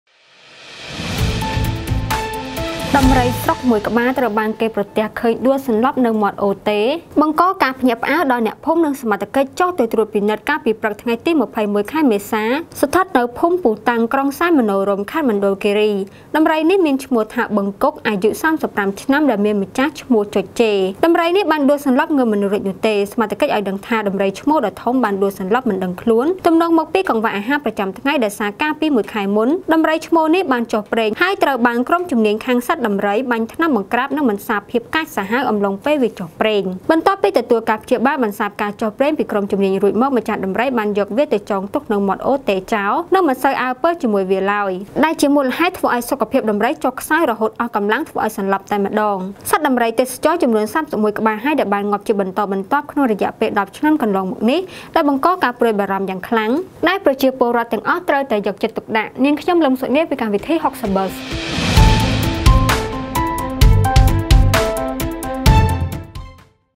ដំរី​ស្រុក​មួយ​ក្បាល ត្រូវ​បាន​គេ​ប្រទះ​ឃើញ​ដួល​សន្លប់​នៅ​មាត់​អូរ​តេ
khmer news,
Cambodia news,
kohsantepheapdaily,
60Buzz